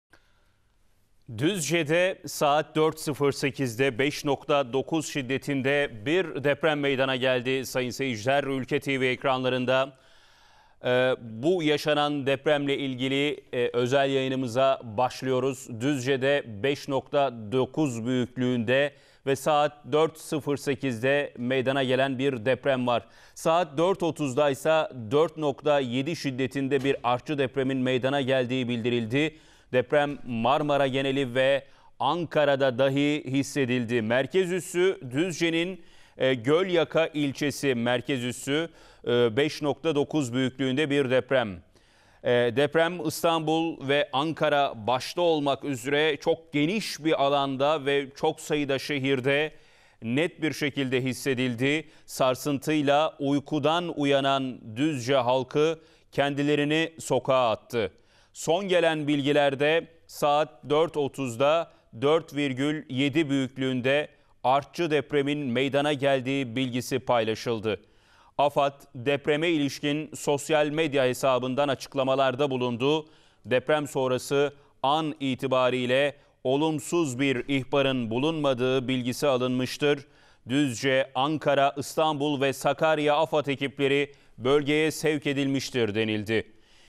SONDAKİKA Düzce'de Korkutan Deprem! İstanbul'da da Hissedildi | İlk Görüntüler
SON DAKİKA HABERİ! Düzce'nin Gölyaka ilçesinde 5,9 büyüklüğünde deprem! İstanbul ve çevre illerde de hissedildi. Deprem sonrası ilk görüntüler...